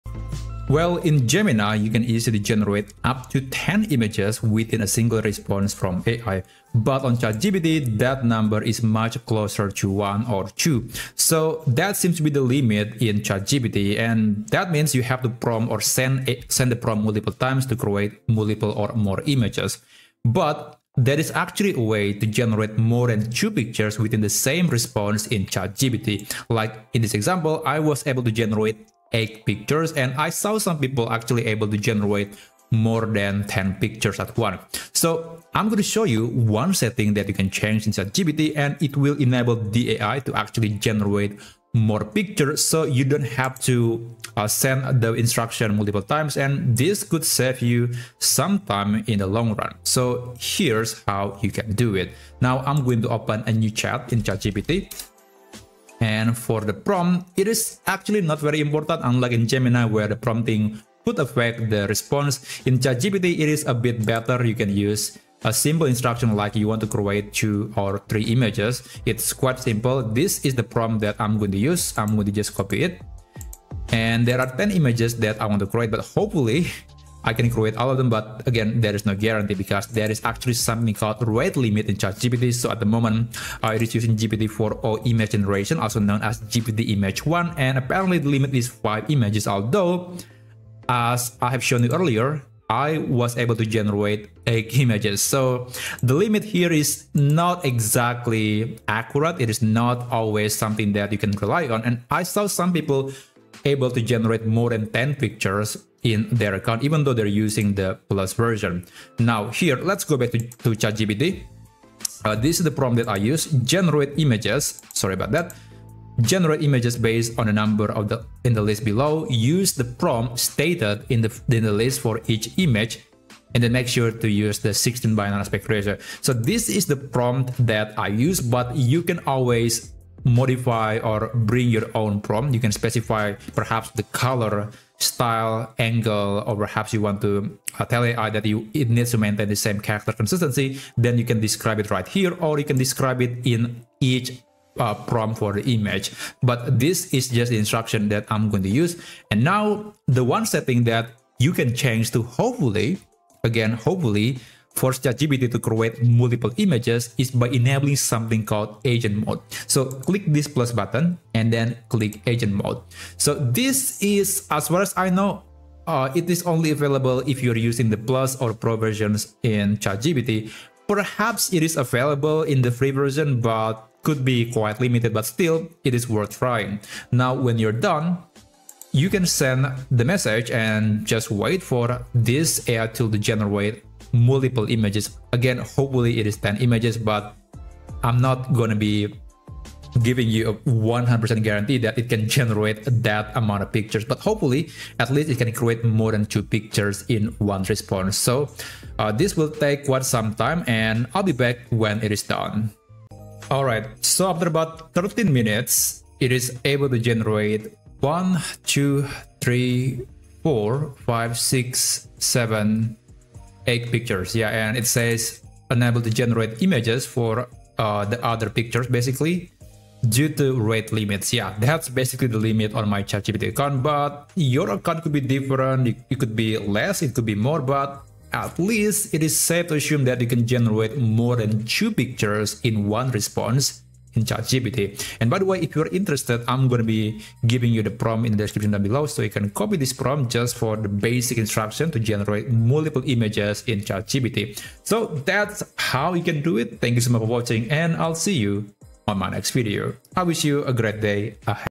How to Batch Generate MULTIPLE Pictures at Once in ChatGPT
👉  In this video, I will show you how to generate multiple images in a single ChatGPT response by enabling one simple setting that allows batch creation. This technique cuts down waiting time between generations and helps maintain visual consistency across characters, scenes, storylines, design variations, and creative projects without typing separate requests each time.

Prompt:
Generate images based on the number in the list below. Use the prompt stated in the list for each image. Make sure to use 16:9 aspect ratio:
1.
2.
3.